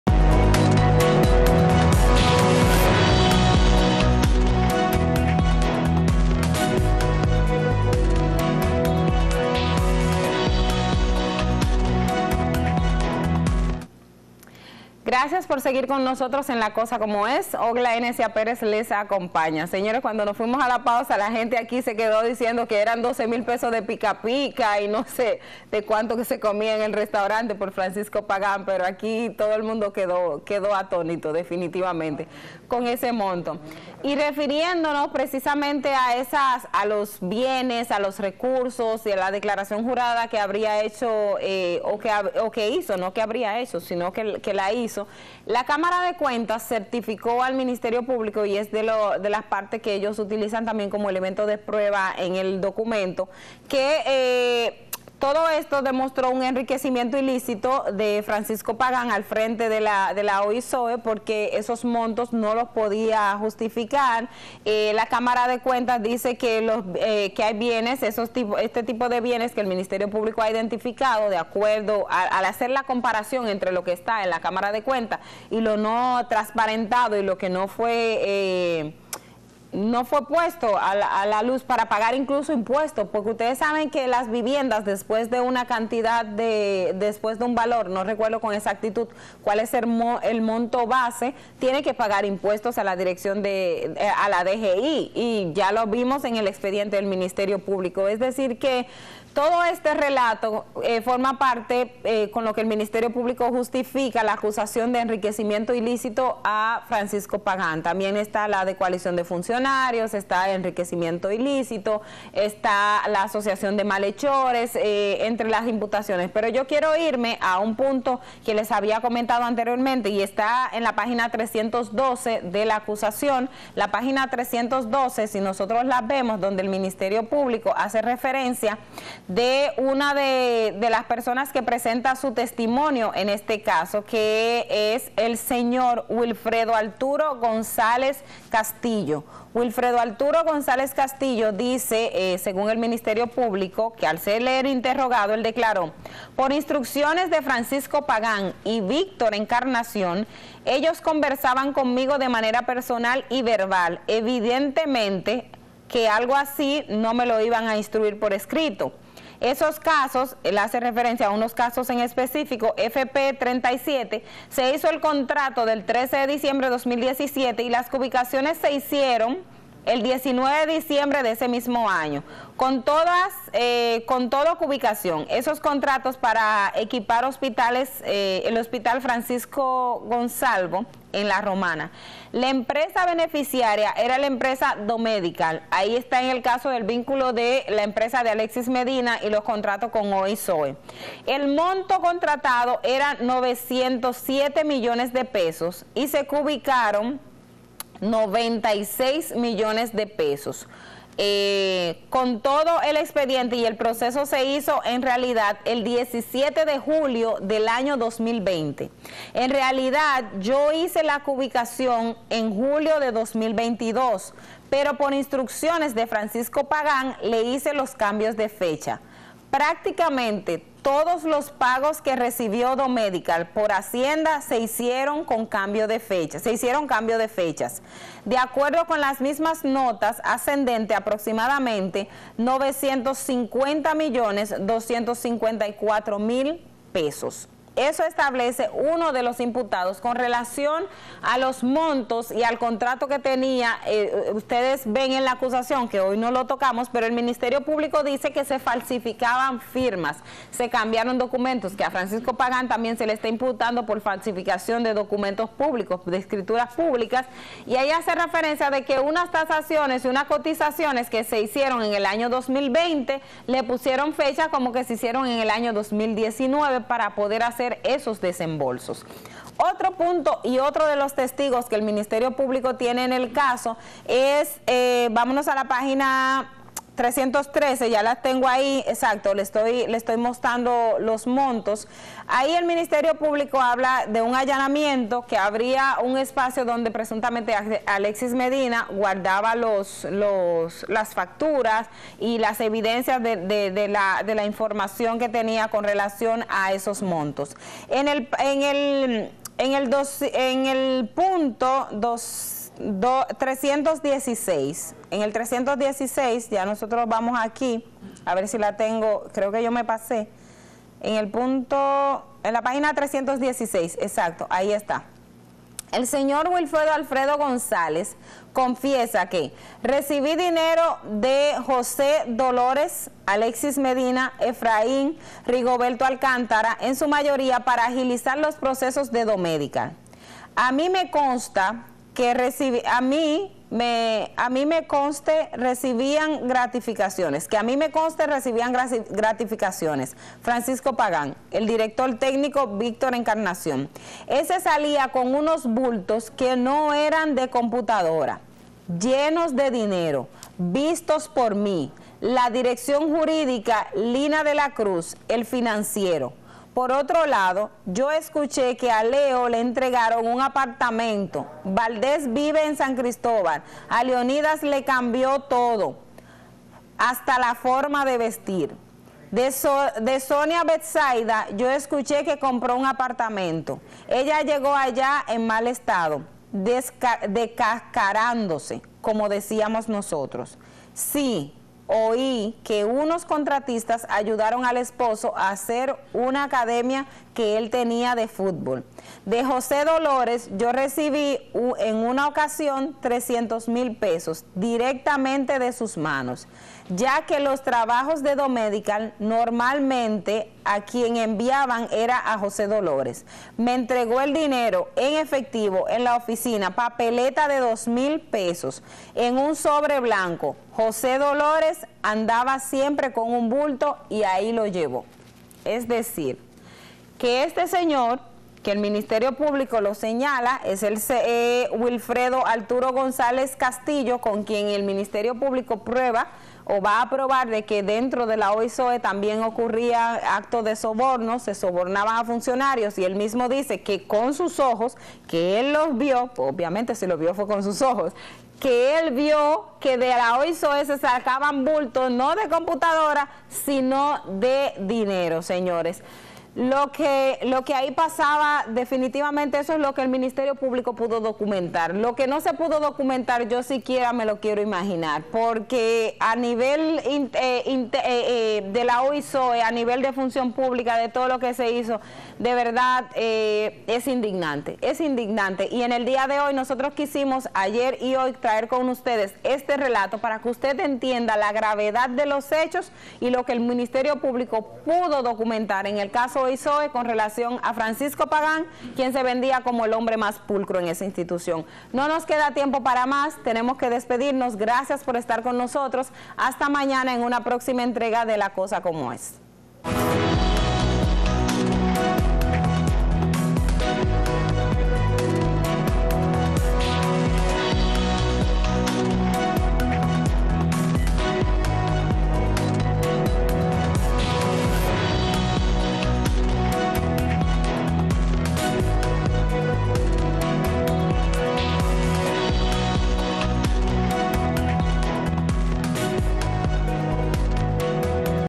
Testigo del MP, declaro que los funcionarios llegaban "descascarándose" a la OISOE y poco eran ricos
Canal Oficial Periodista Edith Febles